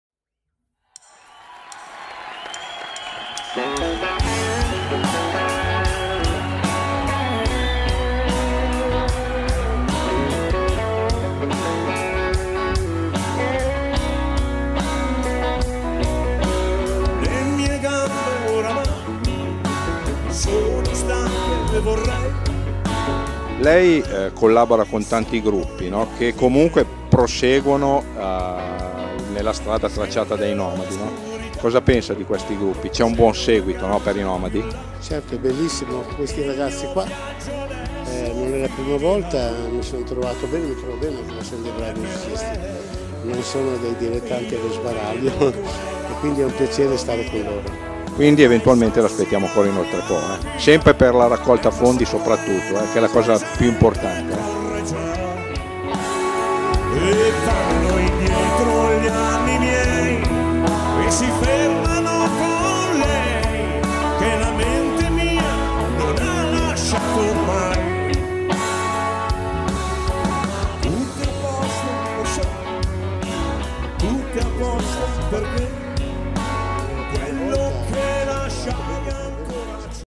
I Senza Patria Nomadi Tribute Band Pavia CASELLE GE 14 LUGLIO
14 Luglio la presa della Bastigla ma a Casella GE Acasella si balla e si canta con i Senza Patria